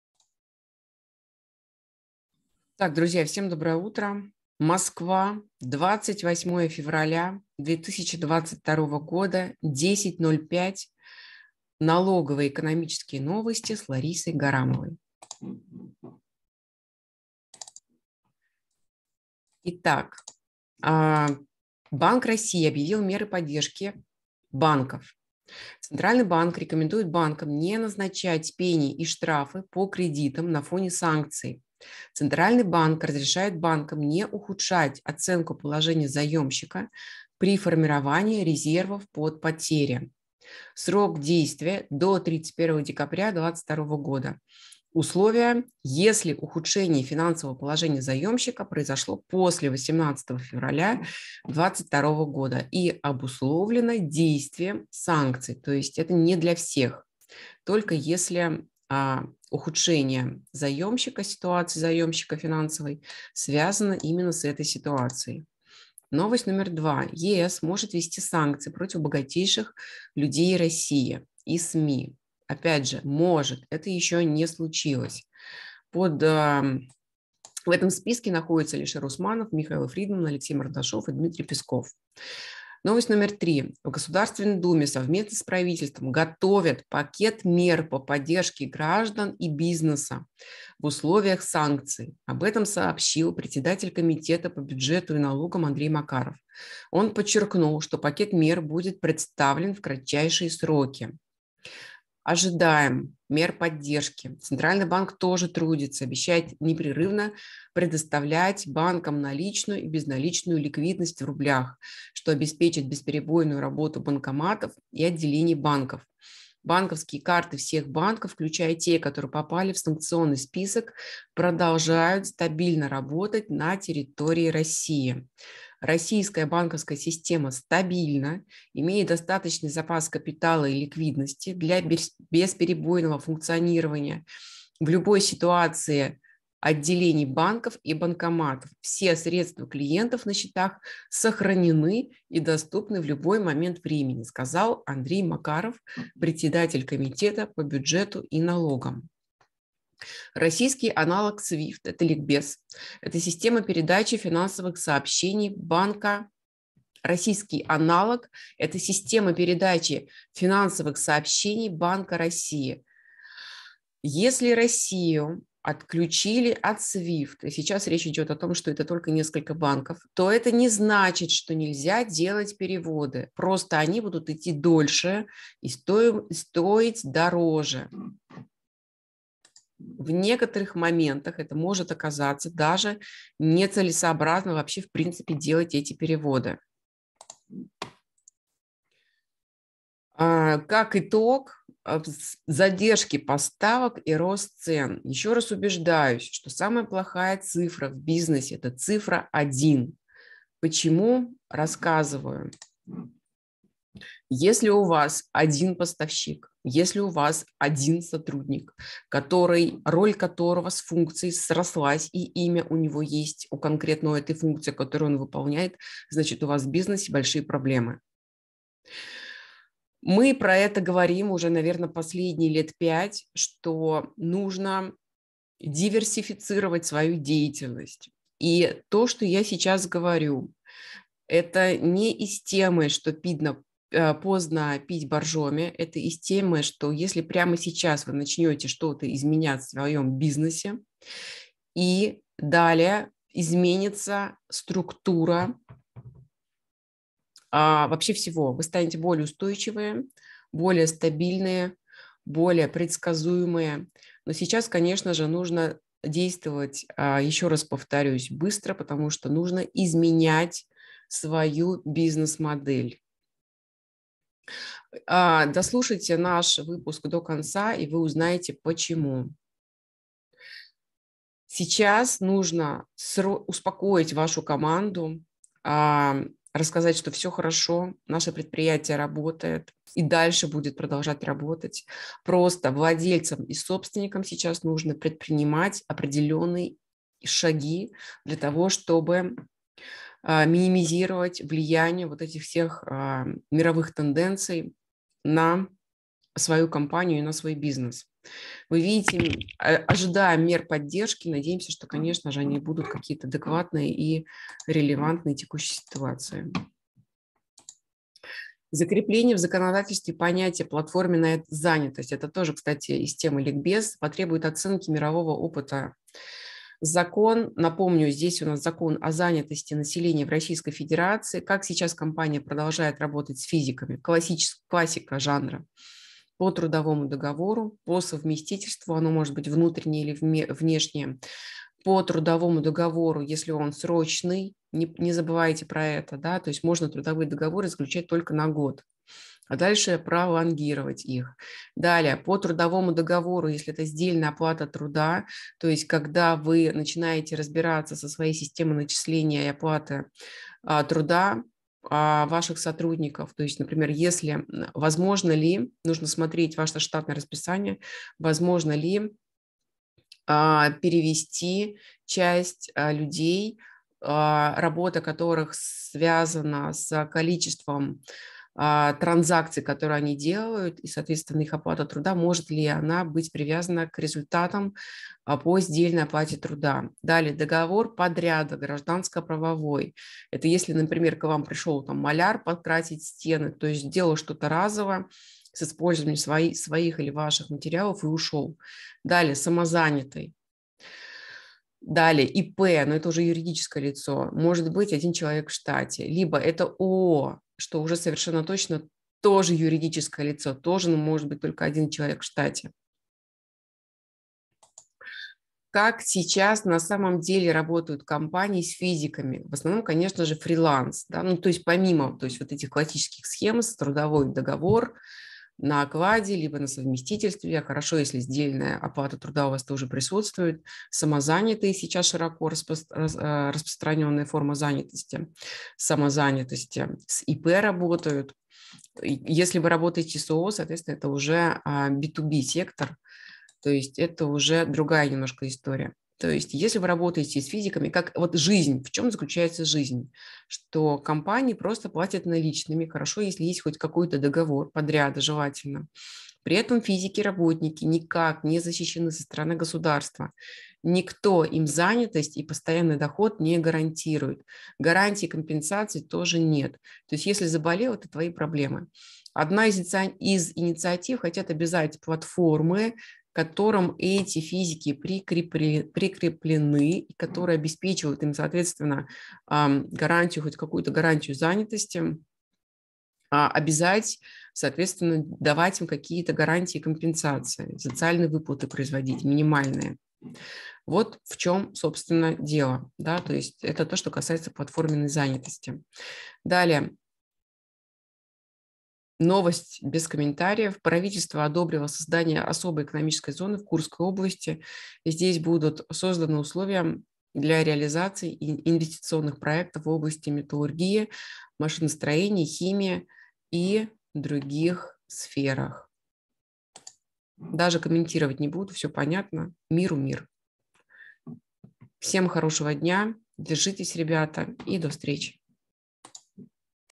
28.02.2022_Налоговые и экономические новости с Ларисой Гарамовой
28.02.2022_Налоговые и экономические новости с Ларисой Гарамовой